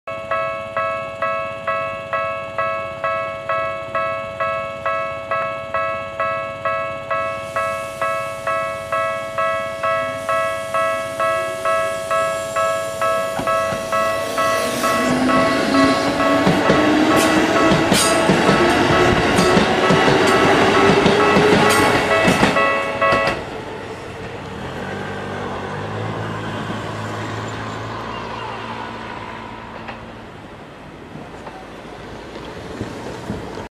京急大師線1500形1505F小島新田行き（正月HM付き） 港町駅発車
京急大師線港町駅を発車する1500形1505Fの普通小島新田行きです。

最近は京急ばかり撮影しているような気がします。まぁ、日中日が昇っている時間帯が短いし、寒いし、必然的に近場での撮影がメインとなるのですが、そうなると東急は結構撮影しているので、京急か小田急かといったところになってしまいます。

今回の映像は何度かアップしていた正月ヘッドマーク付きの大師線の映像の余りです。個人的に1505Fは4連の中で最も撮影している編成なので、できればほとんど遭遇しない1525Fを撮影したかったところでした。しかし1525Fは元日には大師線に入っていたものの、それ以降はほとんど入っていなかったようです。

This is a Local train of Keikyu Daishi Line for Kojimashinden.I recorded this movie at the crossing near Minatocho Station in Kanagawa Prefecture.